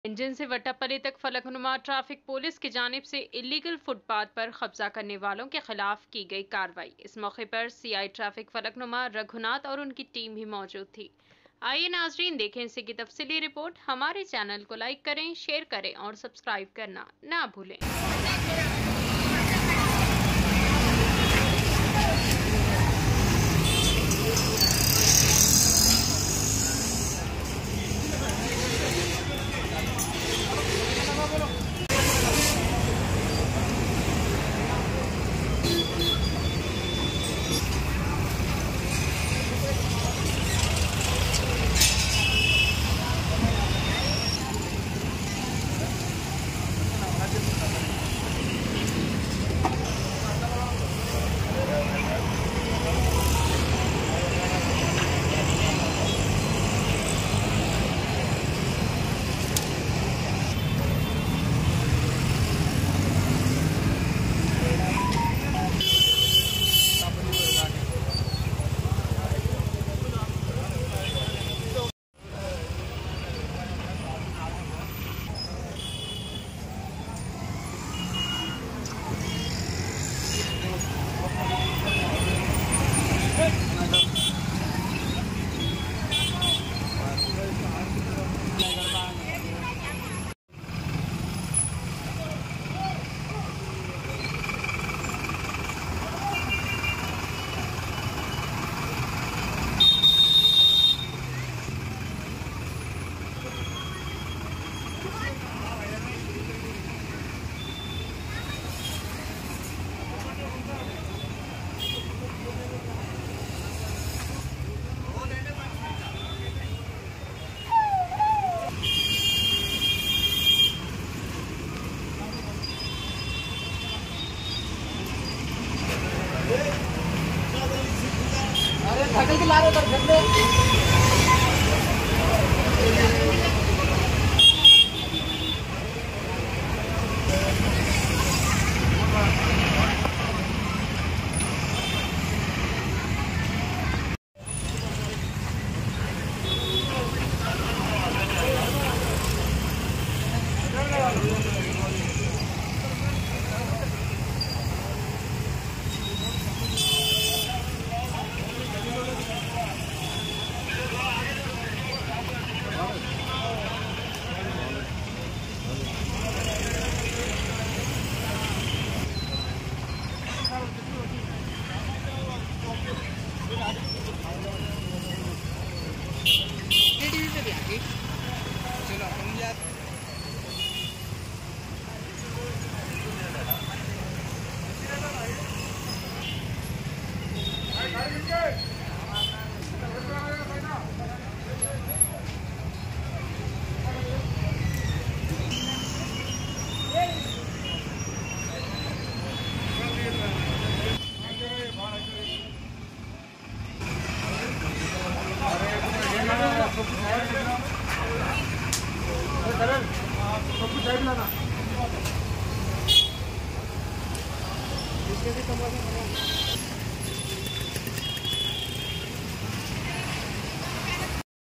Engine Bowli se vettapally tak falaknuma traffic police ki janib se illegal footpath par khabza karn
Engine bowli se vettapally tak falaknuma traffic police ki janib se illegal footpath par khabza karnay walou kay khilaf ki gayi karwahi ..c.i.traffic falknuma raghunath aur un ki team